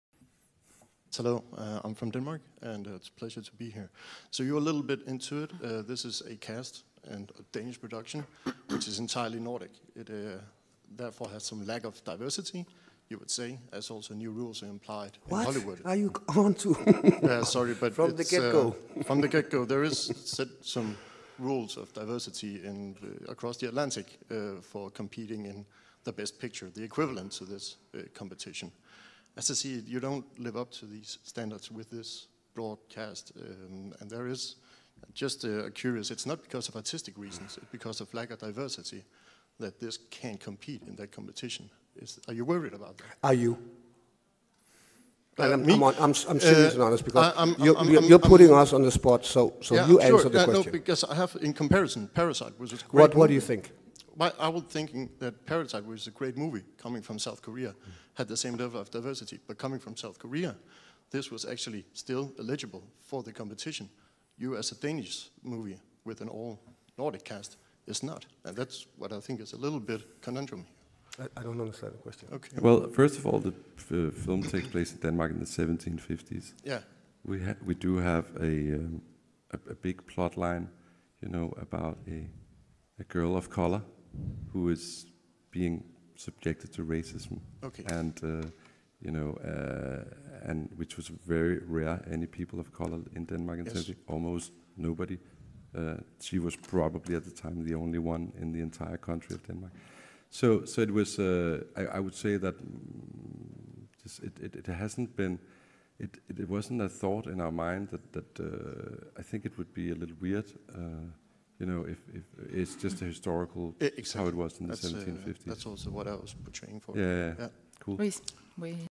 Mads Mikkelsen EXPOSES Journalists Obsession With Hollywood At The Venice Film Festival
The movie is supposed to take place in Denmark in 1750 and the "Journalist" asks about there is NO diversity .

Well there WASNT ANY DIVERSITY in Denmark in the year 1750 DUH ! :P